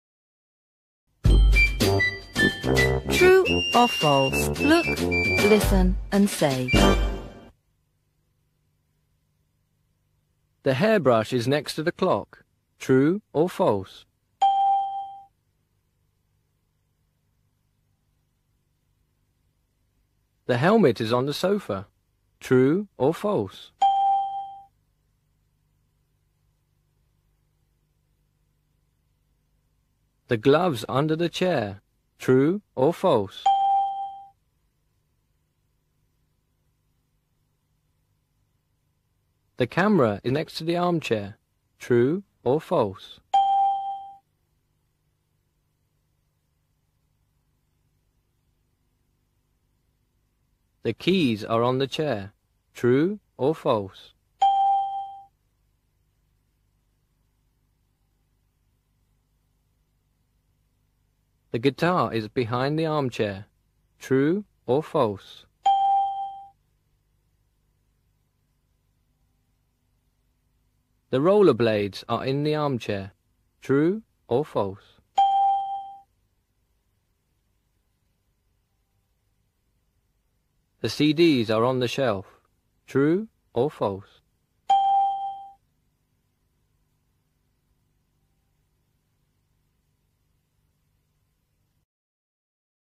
Spotlight 4 True or false? DVD
Английский в фокусе 4 Правда или ложь? ДВД

Student's Book
Учебник

Module 1
Модуль 1

FAMILY & FRIENDS!
СЕМЬЯ И ДРУЗЬЯ!

Look, listen and say.
Посмотри, послушай и скажи.

The hairbrush is next to the clock.
Расческа лежит рядом с часами.
True or false?
Правда или ложь?
The helmet is on the sofa.
Шлем лежит на диване.
True or false?
Правда или ложь?
The gloves are under the chair.
Перчатки лежат под стулом.
True or false?
Правда или ложь?
The camera is next to the armchair.
Камера стоит рядом с креслом.
True or false?
Правда или ложь?
The keys are on the chair.
Ключи лежат на стуле.
True or false?
Правда или ложь?
The guitar is behind the armchair.
Гитара стоит за креслом.
True or false?
Правда или ложь?
The roller blades are in the armchair.
Роликовые лопасти лежат в кресле.
True or false?
Правда или ложь?
The CDs are on the shelf.
Компакт-диски на полке.
True or false?
Правда или ложь?